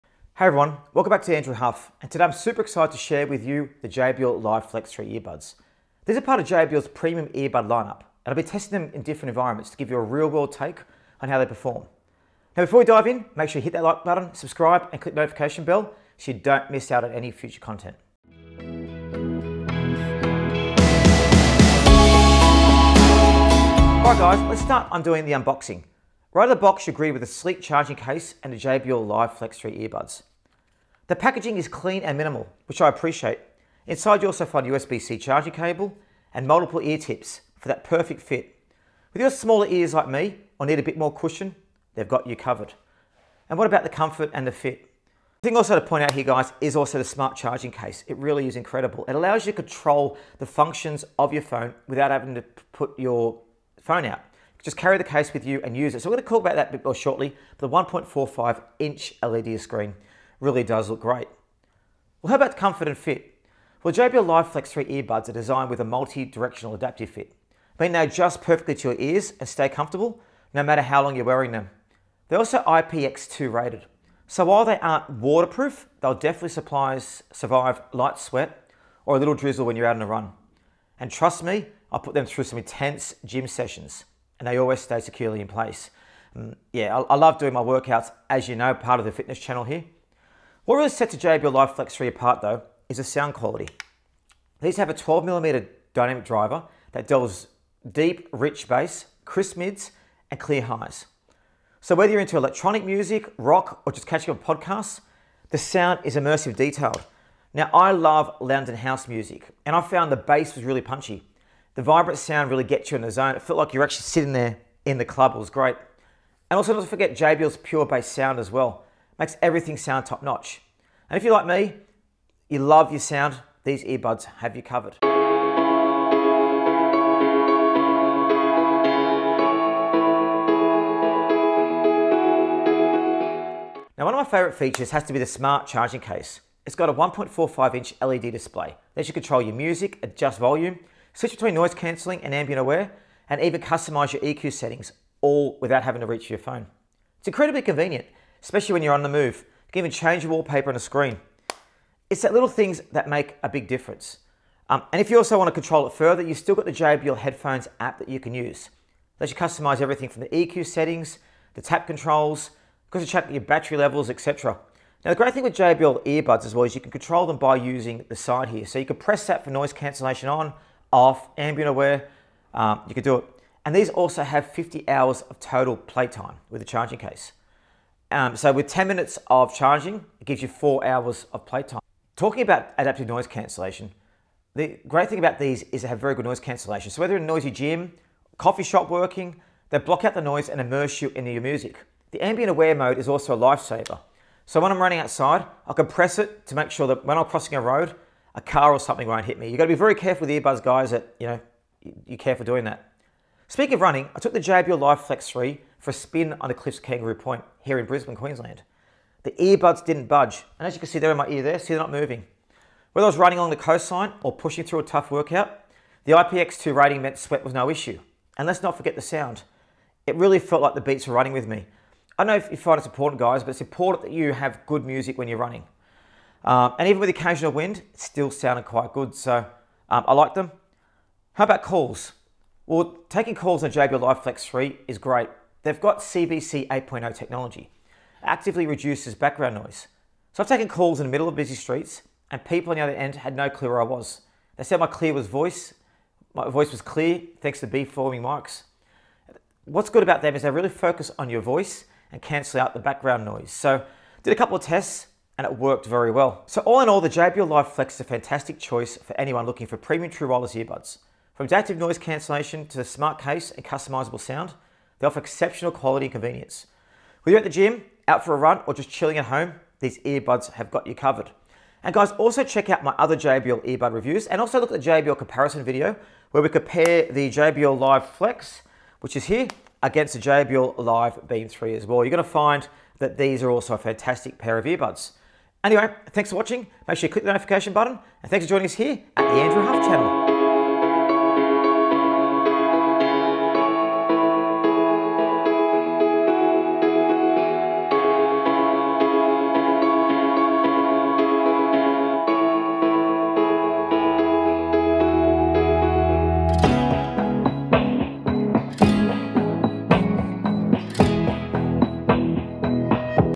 JBL Live Flex 3 Earbuds Review: Unboxing, Sound Test & Real-World Use 🎧
Join me, Andrew Hough, as I dive into a full review of the JBL Live Flex 3 earbuds! From unboxing to real-world testing, we explore every feature of these Live Flex 3 premium wireless earbuds, including sound quality, adaptive noise cancellation, smart charging case, and battery life.

Tested in different environments like the gym, Brisbane’s outdoors, and more, these earbuds prove they’re built to deliver.

Key Features

🎵 12mm Dynamic Drivers for Rich Sound
🔋 50-Hour Battery with Fast Charging
🎚️ Smart Charging Case with LED Display
🌧️ IPX2 Water Resistance for Workouts
📞 Clear Calls with CVC 8.0 Noise Reduction

Video Chapters

0:00 - Intro & Overview
0:30 - Unboxing JBL Live Flex 3
1:00 - Comfort & Fit
1:45 - Sound Quality & Bass Test
2:30 - Smart Charging Case
3:00 - Adaptive Noise Cancellation & Ambient Mode
3:50 - Real-World Testing & Battery Life
4:20 - Call Quality & Final Thoughts